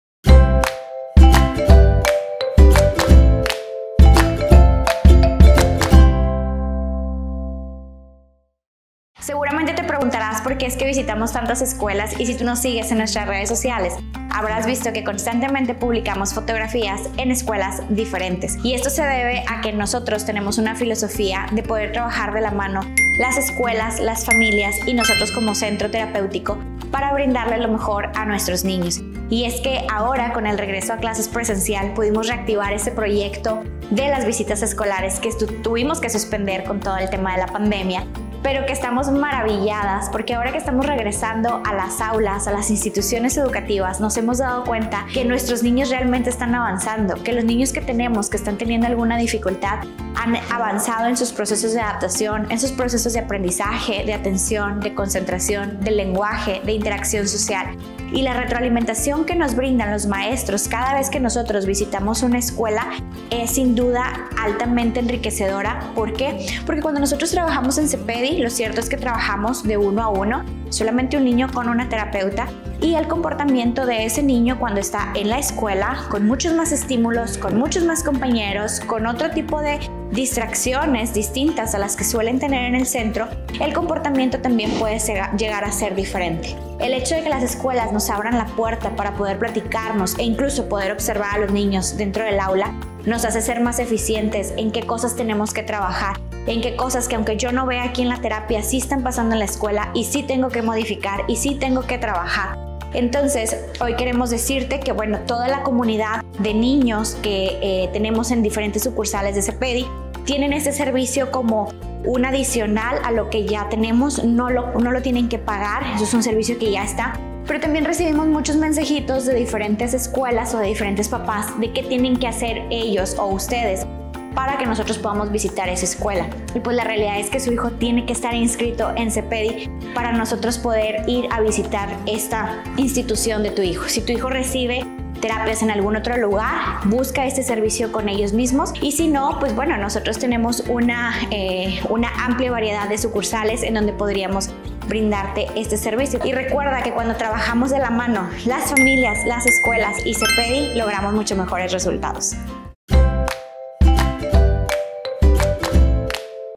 Esta es la razón por la que visitamos tantas escuelas
Recibimos muchos mensajes de papás preguntando que qué deben hacer para que podamos ir a ver a su niño a la escuela... y bueno lo primero es necesario que su hijo (a) esté tomando terapia con nosotros para poder brindarle este servicio.

También hay escuelas que nos escriben pidiendo que vayamos a ver a algunos de sus alumnos, y la respuesta es la misma, dichos niños tendrían que estar en terapia con nosotros para brindarles ese servicio con previa autorización de los papás.

Y una vez teniendo esto como premisa, lo siguiente es platicarles que estas visitas nos sirven para poder trabajar en equipo las ESCUELAS + FAMILIAS + CEPEDI y en conjunto tomar las mejores decisiones para apoyar a los niños a superar las dificultades.